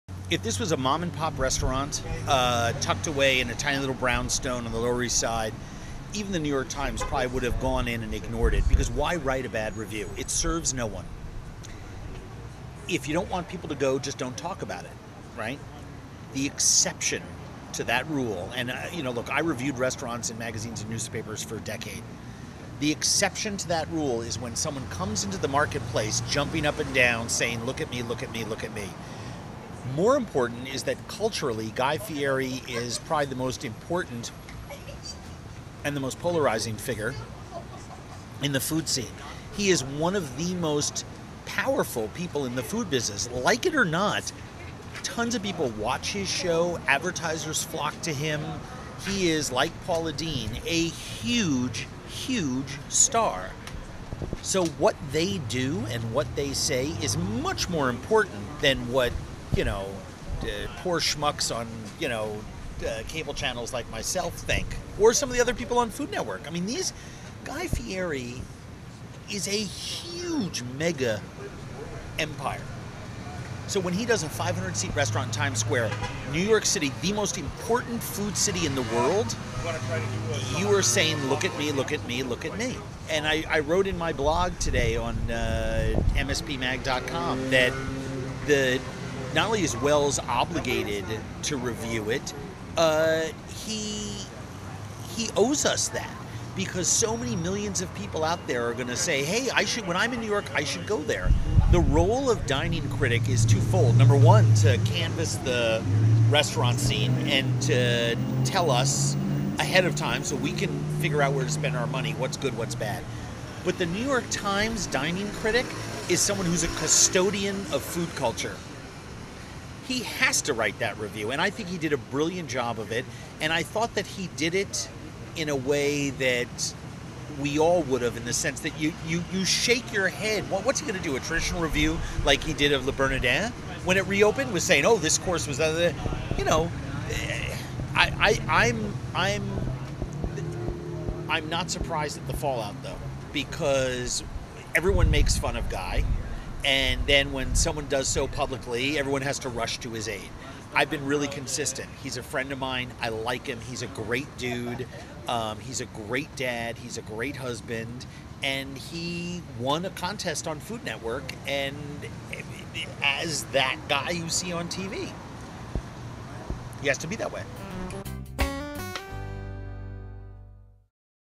Andrew Zimmern Talks About Guy Fieri
Andrew Zimmern shares his thoughts on Guy Fieri and the celebrity's New York City restaurant that received poor reviews in recent weeks. Star-Advertiser video by Jason Genegabus.